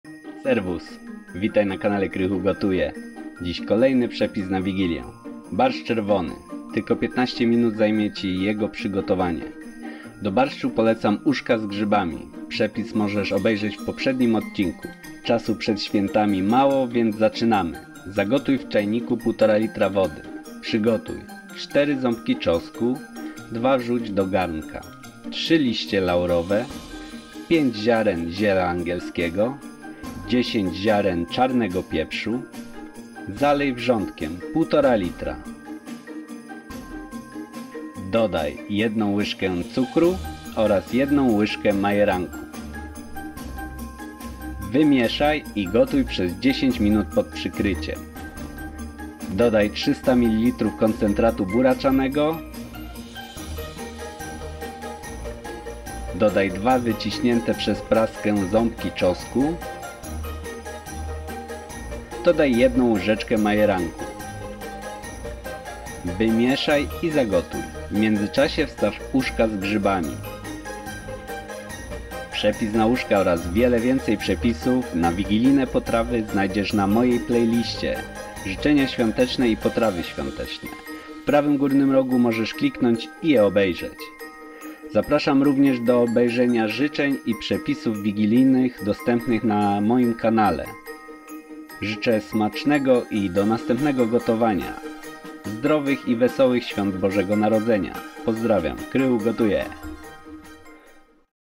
Barszcz czerwony wigilijny w 15minut. Bardzo pyszny i szybki sposób na barszcz na Wigilię z uszkami.
Barszcz czerwony wigilijny przygotujesz według tego przepisu w 15minut. Barszcz czerwony jest bardzo pyszny, idealny do uszek z grzybami jako pierwsze danie wigilijne. Ta potrawa wigilijna to jakby dwie potrawy wigilijne w jednym. W czasie przedświątecznym zawsze brakuje czasu na przygotowanie potraw. Dlatego mój szybki sposób na barszcz na Wigilię z uszkami. Przepis na uszka z grzybami możesz zobaczyć w poprzednim odcinku. Uszka do barszczu wigilijnego przygotowuje dużo wcześniej i mrożę. Natomiast barszcz czerwony przygotowuje ze sprawdzonego koncentratu buraczanego z butelki w dniu 24.12, czyli w dzień kolacji wigilijnej.
Do przygotowania barszczu czerwonego potrzebować będziesz:
♦ 4 ząbki czosnku (dwa wrzuć do garnka)
♦ 3 liście laurowe
♦ 5 ziaren ziela angielskiego,
♦ 10 ziaren czarnego pieprzu,
♦ 1 łyżkę cukru
♦ 1 łyżka majeranku,
To podstawa do barszczu wigilijnego. Zalewam wszystkie składniki 1,5literem wrzącej wody i gotuje przez 10minut pod przykryciem.
Dodaje:
♦ 300ml koncentratu buraczanego
♦ 2 wyciśnięte przez praskę ząbki czosnku,
♦ 1 łyżkę majeranku,
Oczywiście jak ktoś lubi bardziej wyrazisty smak, można dodać jeszcze świeżo zmielonego pieprzu i octu. Natomiast ta wersja barszczu jest według mnie idealna i zadowoli większość.
Zapraszam również do obejrzenia innych filmów na potrawy wigilijne.
Pozdrawiam.
KrychuGotuje.